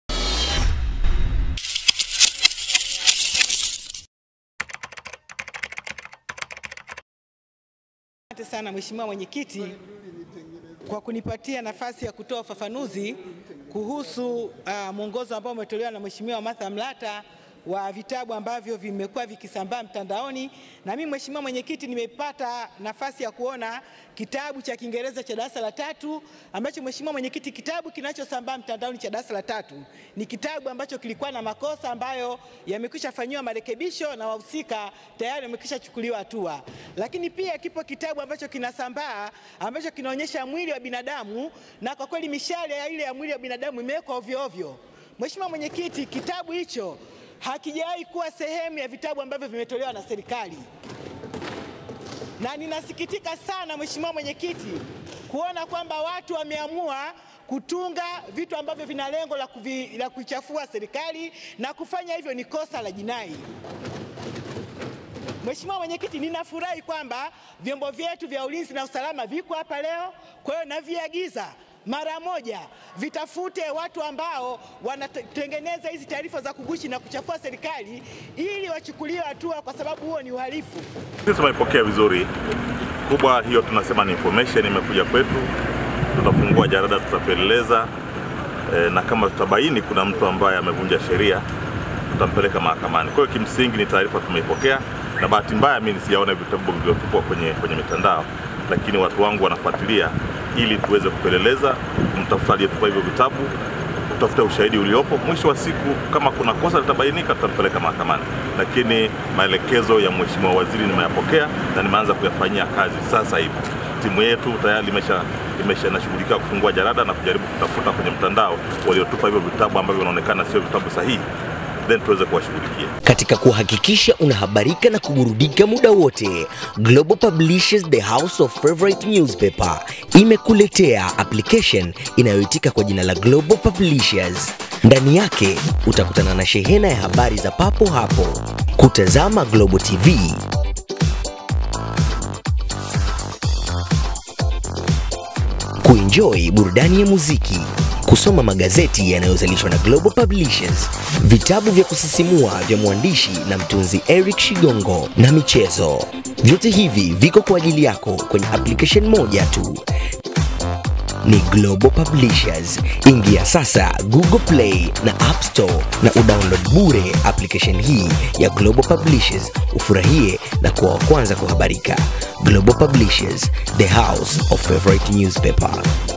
NDALICHAKO BUNGENI: Atoa Agizo Hili kwa IGP Sirro!
Waziri wa Elimu, Profesa Joyce Ndalichako, ametoa ufafanuzi Bungeni kuhusiana na taarifa za vitabu vinavyosambaa mitandaoni kikiwemo kitabu cha kiingereza cha darasa la tatu kilichokuwa na makosa pamoja na kile kinachonyesha mchoro wa picha ya mwili wa binadamu ambao umekosewa.

Profesa Ndalichako amesema kitabua hicho sio sehemu ya vitabu vilivyotolewa na serikali na ametoa agizo kwa vyombo vya usalama kufanya uchunguzi kumbaini aliyetunga  kitabu hicho na kumchukulia hatua.

Mkuu wa jeshi la polisi nchini, IGP Simon Sirro, amesema amelipokea agizo la Waziri Ndalichako na tayari wameshaanza kulifanyia kazi.